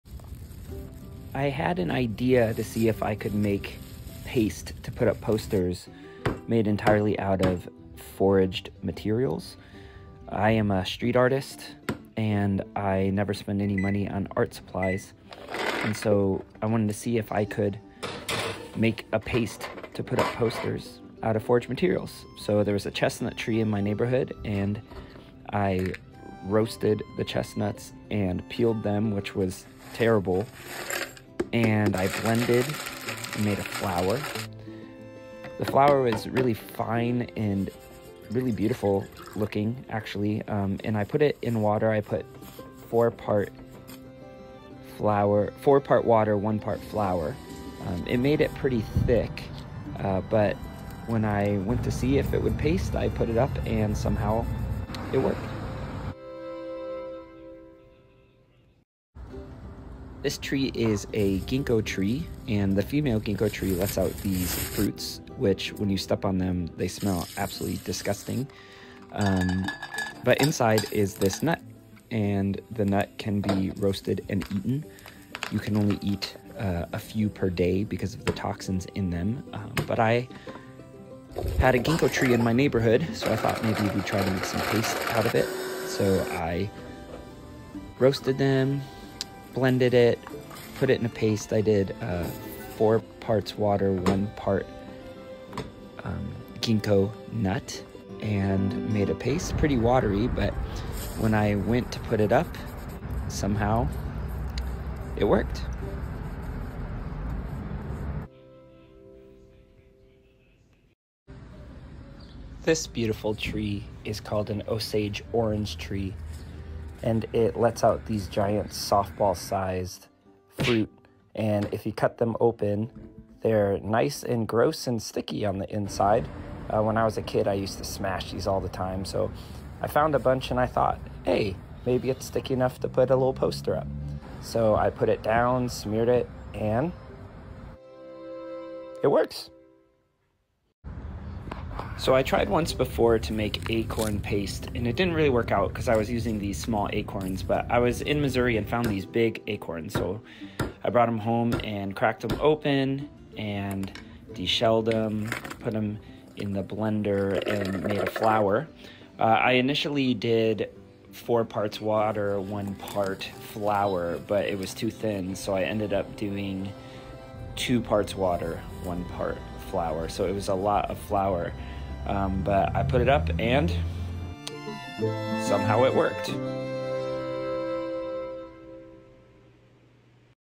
Foraged wheatpaste made from chestnut, ginkgo, Osage orange and acorn.
I am a street artist and I don’t spend any money on art supplies. So I wanted to see if I could make a paste (glue) out of foraged materials.